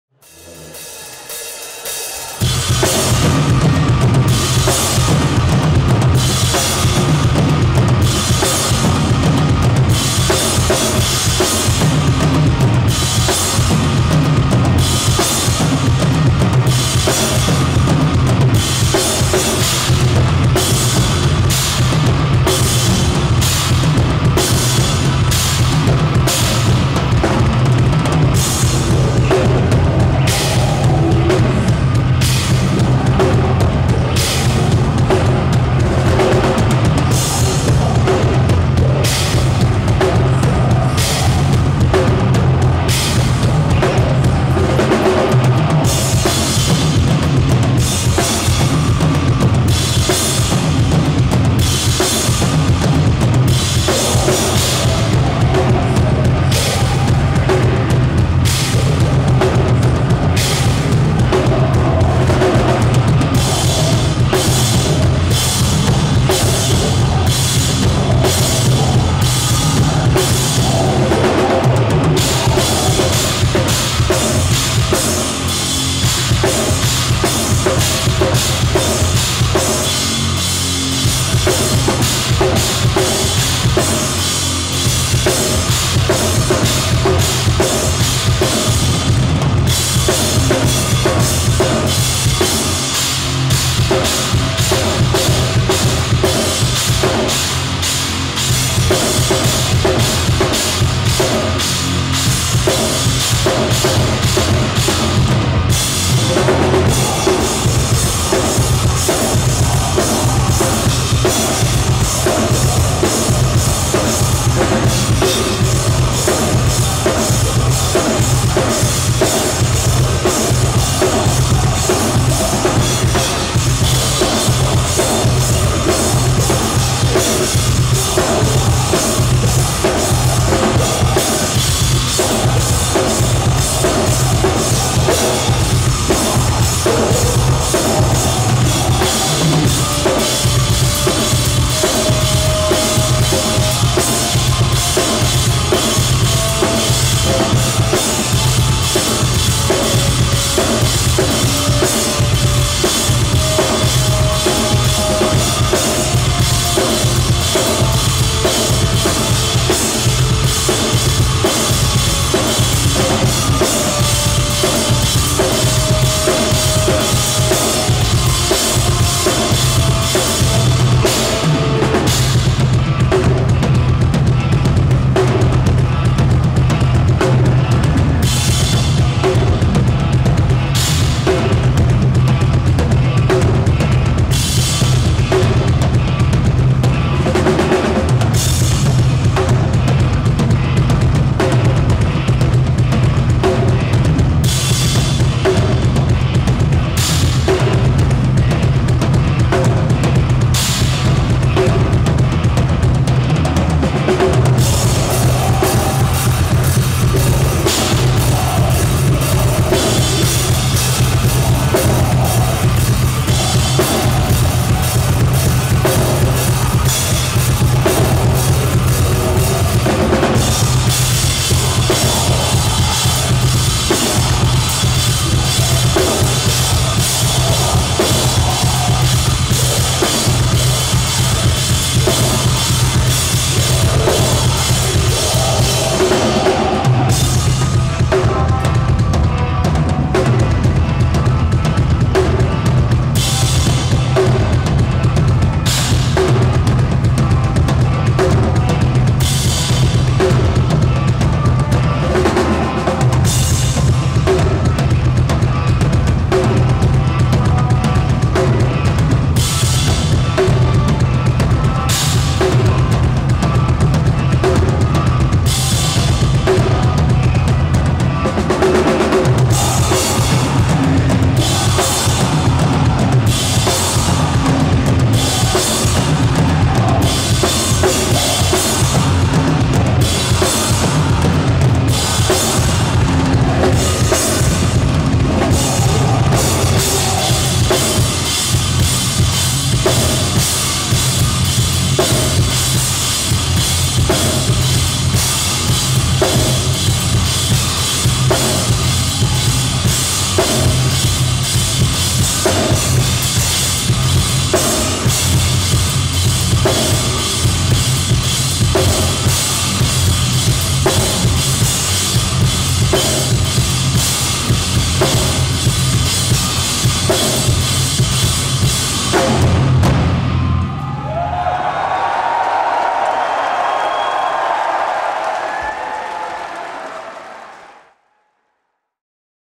MESHUGGAH - Tomas Haake - Drumming Footage - New England Metal Fest (OFFICIAL)
Taken from the The Palladium - Worcester, MA on April 27, 2008